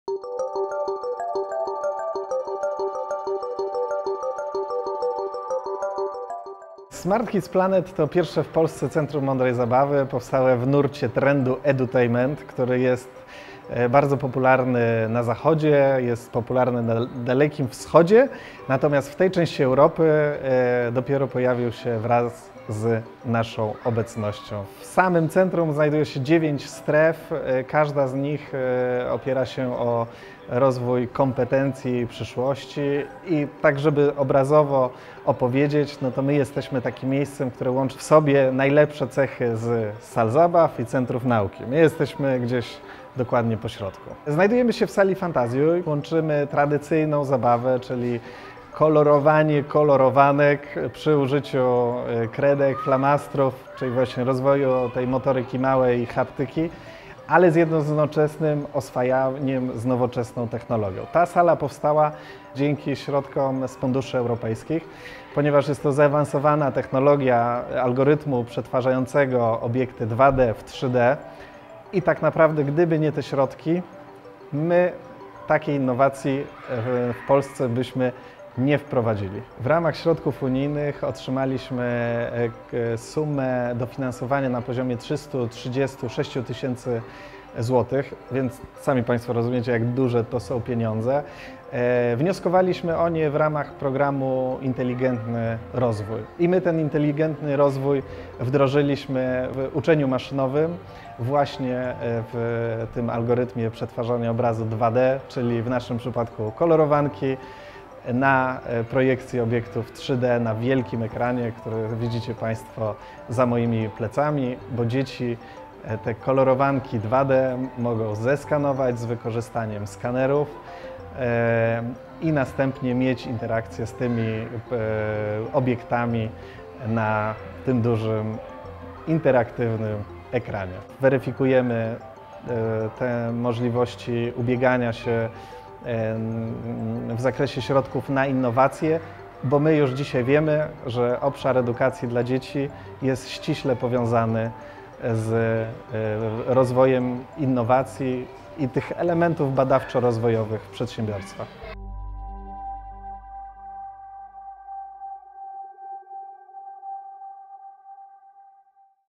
Już prawie pół miliona dzieci odwiedziło centrum Smart Kids Planet dofinasowane przez UE
Smart Kids Planet w Fabryce Norblina na warszawskiej Woli to unikatowe miejsce łączące zabawę z interaktywną nauką oraz beneficjent Funduszy Europejskich. Ponad 336 tys. złotych dofinansowania pozyskanego dzięki wsparciu z Programu Inteligentny Rozwój (POIR), przekazanemu przez Polską Agencję Rozwoju Przedsiębiorczości (PARP), pozwoliło stworzyć w centrum nową, multimedialną przestrzeń - Strefę Fantazjuj.